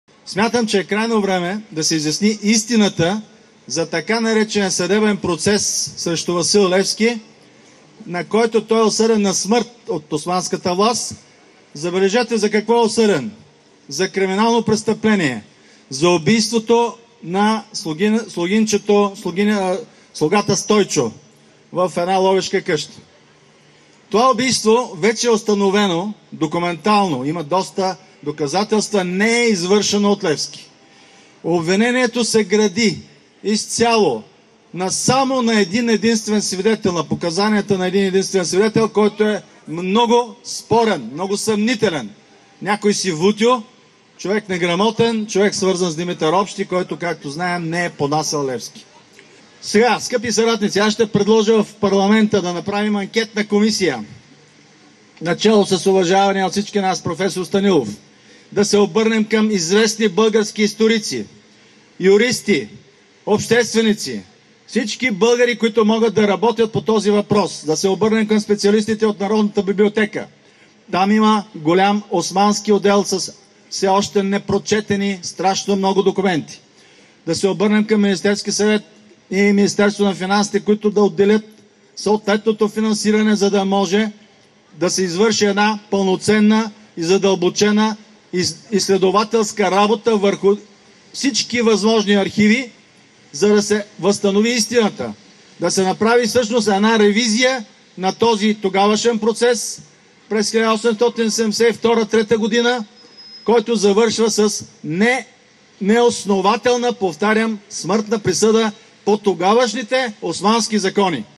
Сидеров: Ще направим анкетна комисия, която обстойно да проучи съдебния процес срещу Левски /19....
Сидеров: Ще направим анкетна комисия, която обстойно да проучи съдебния процес срещу Левски /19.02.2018 г./

Смятам, че е крайно време да се изясни истината за така
наречения съдебен процес срещу Васил Левски, на който е осъден на смърт от Османската власт. Ще предложа в НС да бъде съставена анкетна комисия, за да се извърши пълноценен и задълбочен изследователски анализ. Предлагам начело
на комисията да е народният представител от АТАКА уважаван проф. Станислав Станилов и към нея да се присъединят известни историци, юристи, общественици и всички българи, които могат да работят по този въпрос. Това заяви лидерът на ПП АТАКА Волен Сидеров пред паметника на Апостола по повод 145 години от неговото обесване